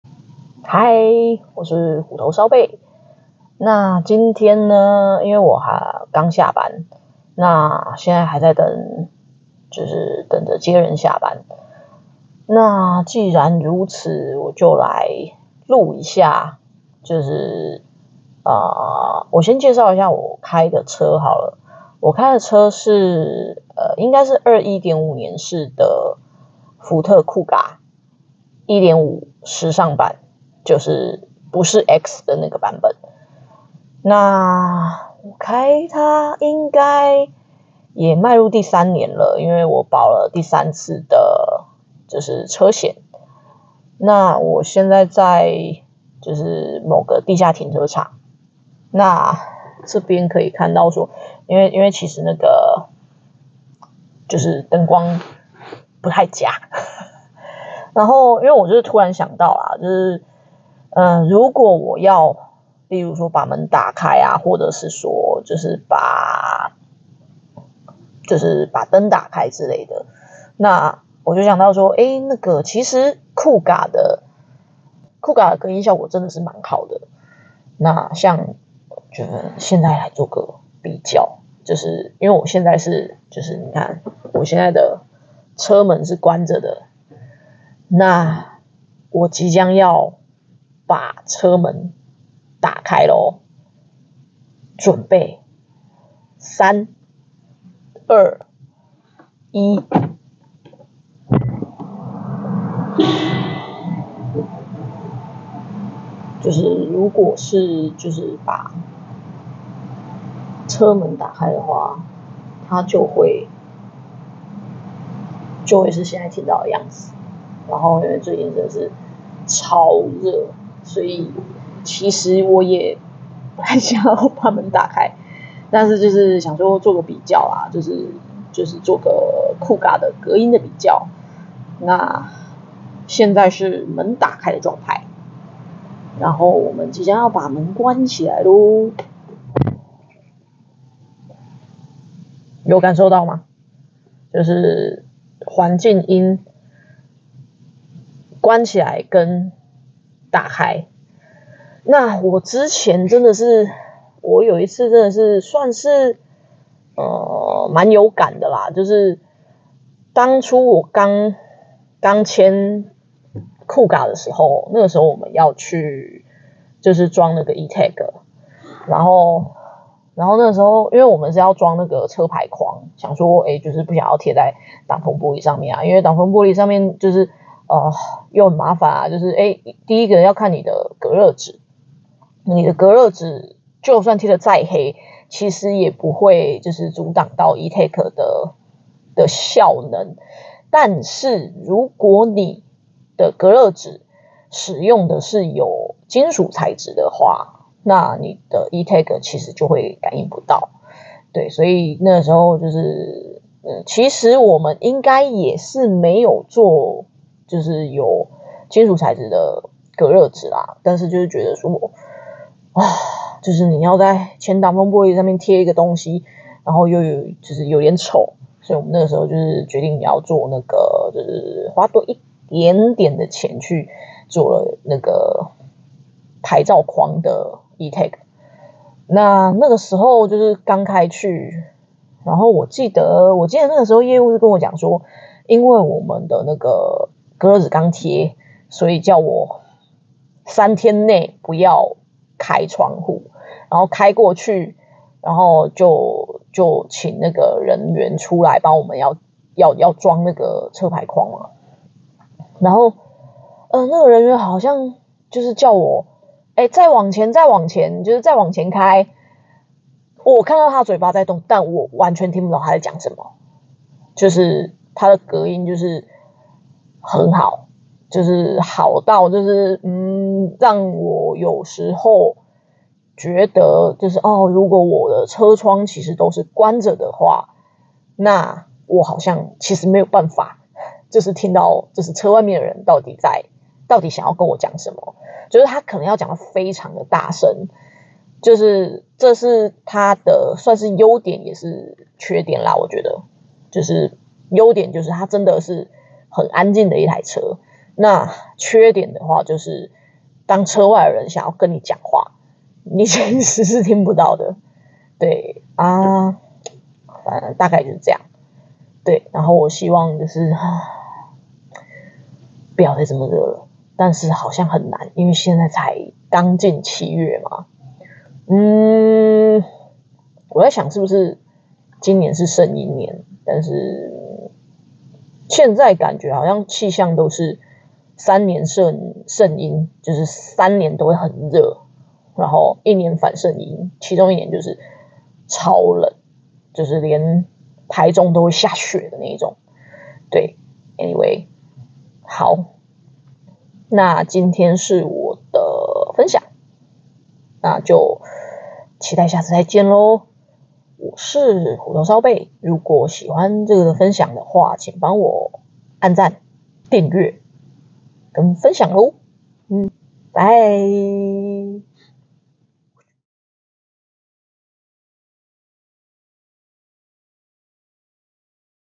實際測試系列 - 福特Kuga靜謐性測試
突然想起之前去裝eTag牌照框
然後店員叫我往前開
我一直聽不到他講話

每次去加油站洗車也是
很怕聽不到店員說話

錄影設備：Samsung S22 plus
剪輯設備：Samsung Tab S9
剪輯軟體：LumaFusion (Android Version)